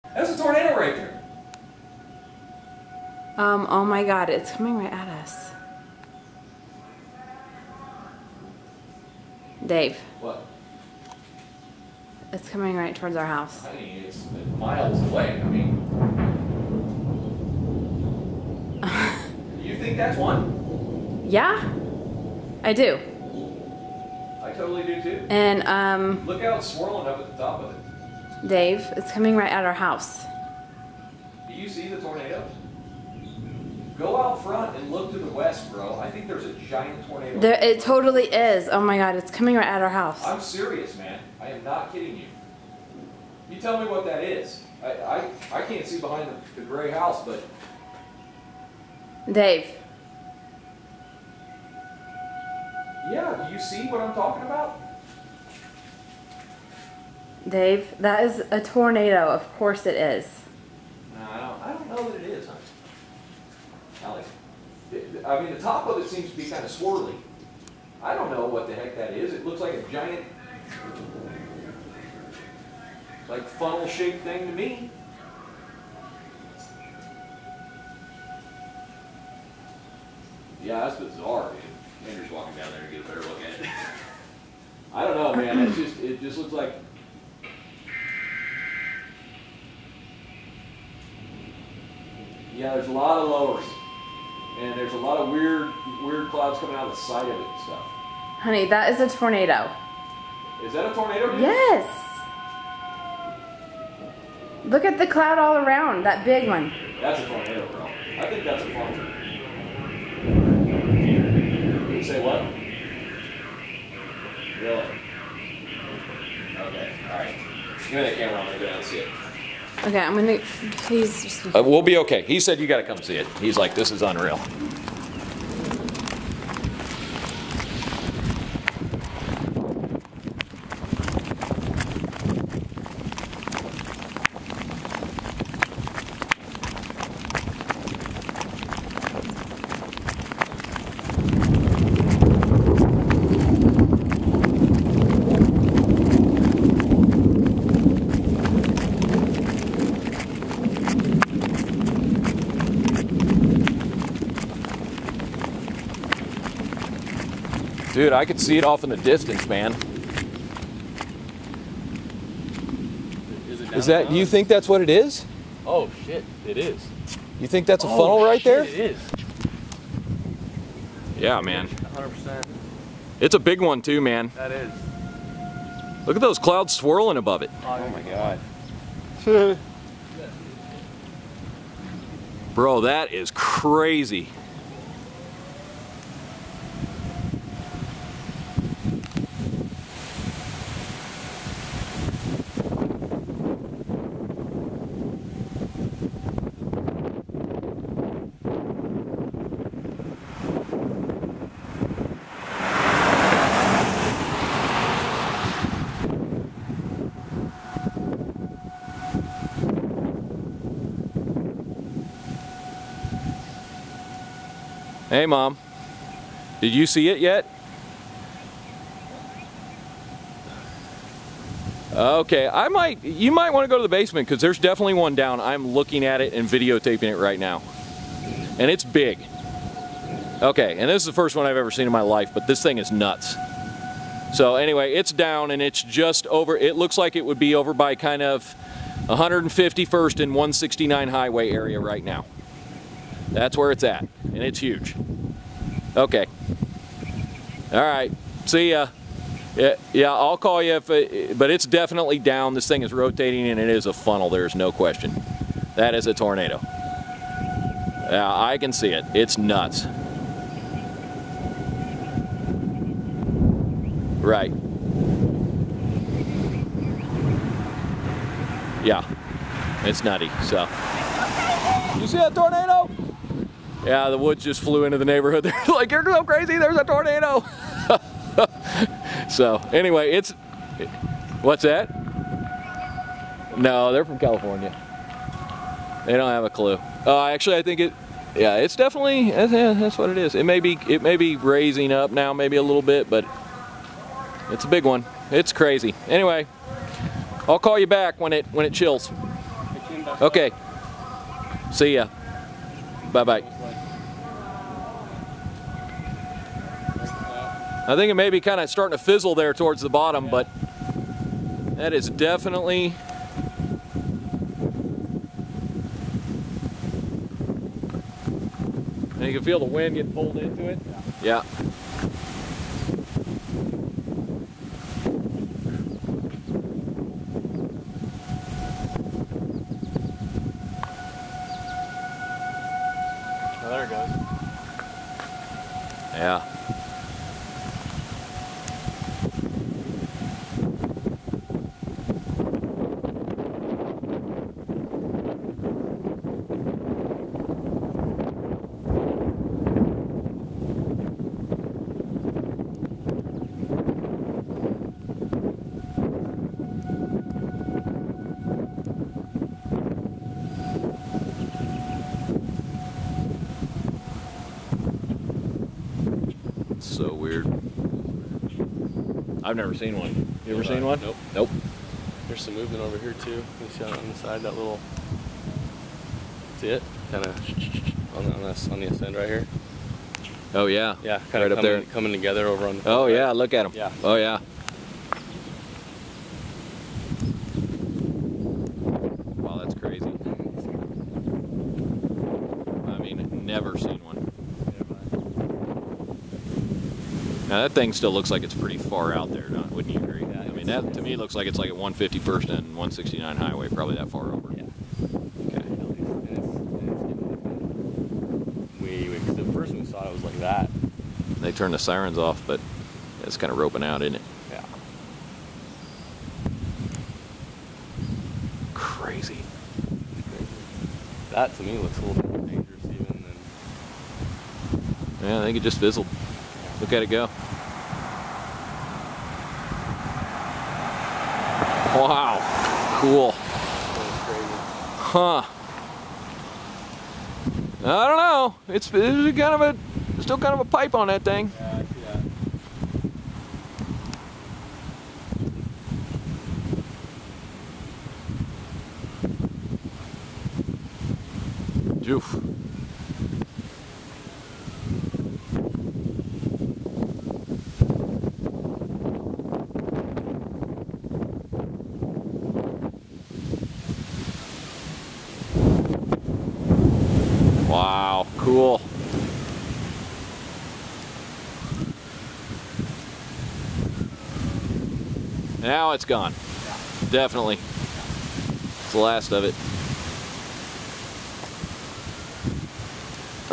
Olathe Kansas Tornado Funnel May 6th 2012
We videotaped this from our front driveway and neighborhood!! We live near 151st and Blackbob in Olathe